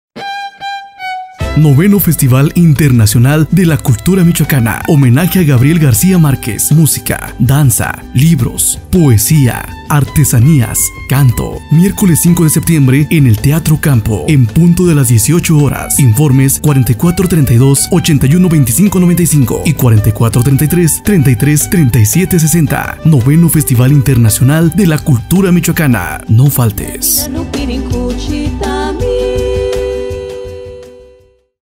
9no Festival de la Cultura Michoacana
No faltes a esta gran fiesta de cultura y tradición este próximo 5 de septiembre a las 18:00 horas en la Teatro Morelos, Morelia, Michoacán, México. Venta de boletos, el día del evento, sin numeración.